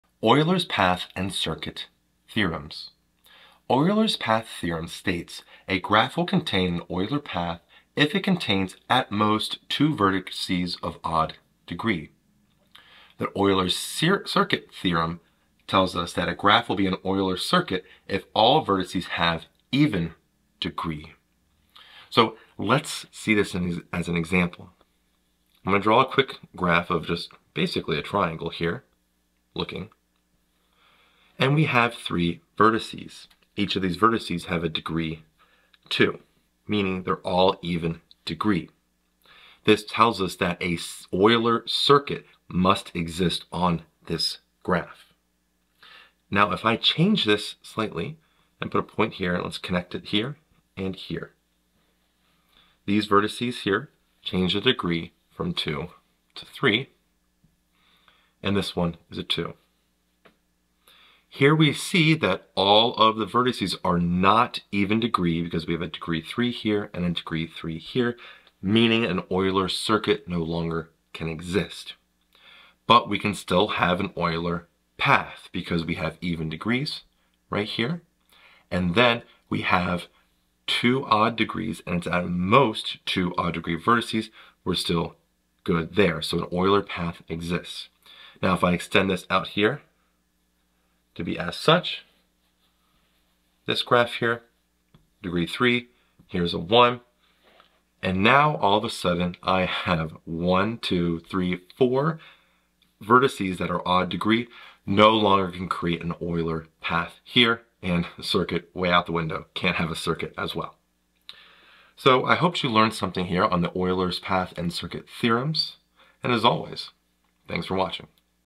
Euler's Path and Circuit Theorems Explained | Graph Theory Basics
Explore essential concepts in graph theory with our video, "Euler's Path and Circuit Theorems Explained | Graph Theory Basics". This comprehensive guide illuminates Euler's Path and Circuit Theorems, key insights that are foundational to understanding how paths and circuits function within various types of graphs. Ideal for students, educators, and professionals, this video explains the theorems in an accessible manner, provides historical context, and demonstrates their application through practical examples. Whether you're just starting out or brushing up on your knowledge, this video is a valuable resource.